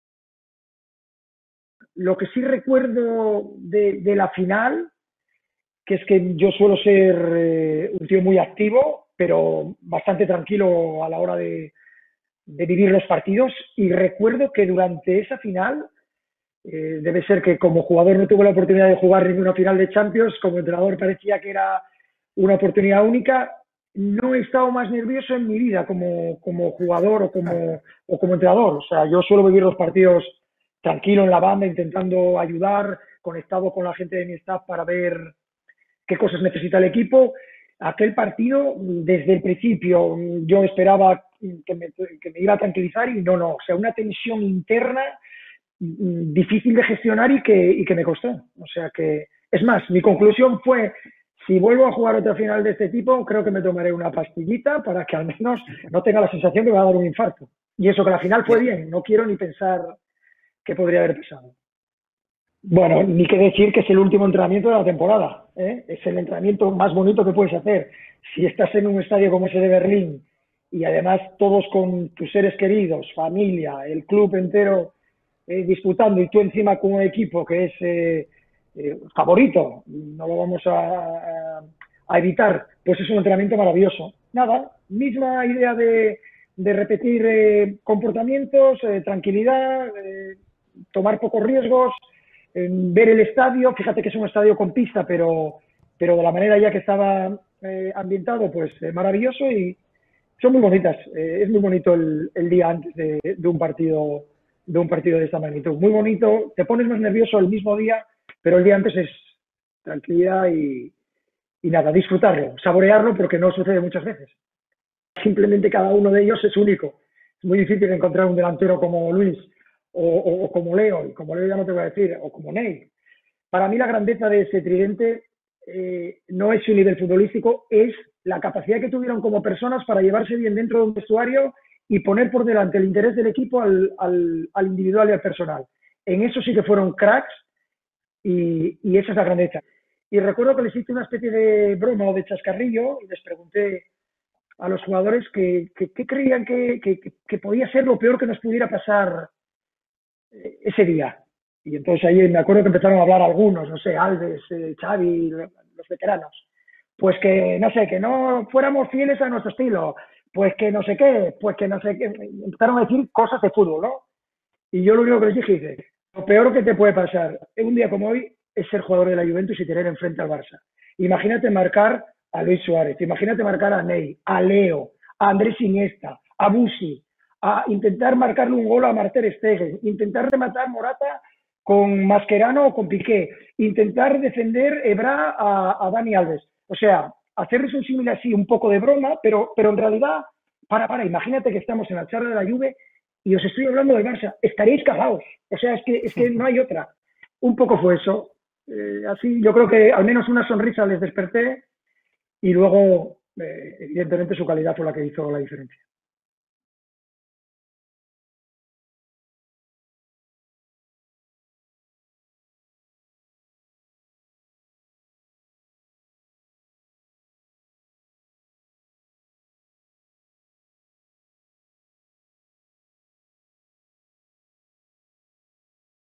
Luis Enrique remembers Messi, Suárez & Neymar "They put the team before themselves"
Luis Enrique looks back at the historic victory over Juventus in Berlin and remembers how Messi, Suárez and Neymar worked as one of history's greatest attacking sides.